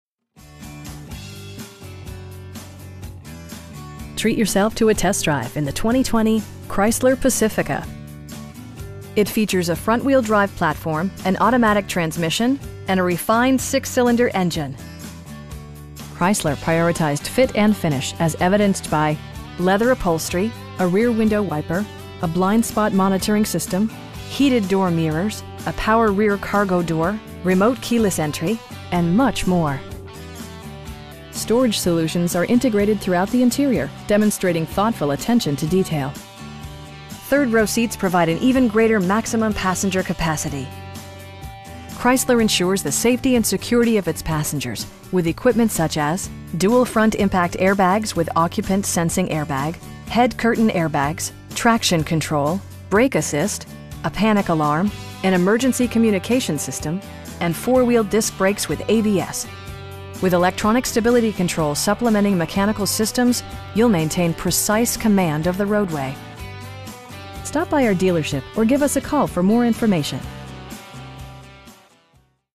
2020 Chrysler Pacifica TOURING L in Glenview, IL 60026-8041
2800 Patriot Blvd.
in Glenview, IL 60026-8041

Treat yourself to a test drive in the 2020 Chrysler Pacifica. It features a front-wheel-drive platform, an automatic transmission, and a refined 6 cylinder engine. Chrysler prioritized fit and finish as evidenced by: leather upholstery, a rear window wiper, a blind spot monitoring system, heated door mirrors, a power rear cargo door, remote keyless entry, and much more. Storage solutions are integrated throughout the interior, demonstrating thoughtful attention to detail. Third row seats provide an even greater maximum passenger capacity. Chrysler ensures the safety and security of its passengers with equipment such as: dual front impact airbags with occupant sensing airbag, head curtain airbags, traction control, brake assist, a panic alarm, an emergency communication system, and 4 wheel disc brakes with ABS. With electronic stability control supplementing mechanical systems, you'll maintain precise command of the roadway. We pride ourselves in the quality that we offer on all of our vehicles. Stop by our dealership or give us a call for more information.